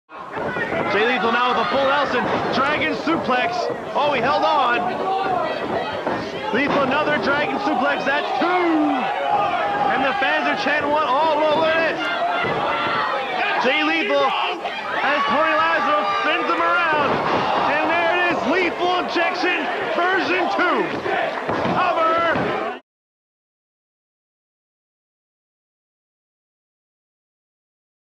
Jay Lethal with That's All She Wrote!
Jay Lethal lands two Roling Dragon Suplexes on Tony Lazaro, before dropping him on his neck with That's All She Wrote!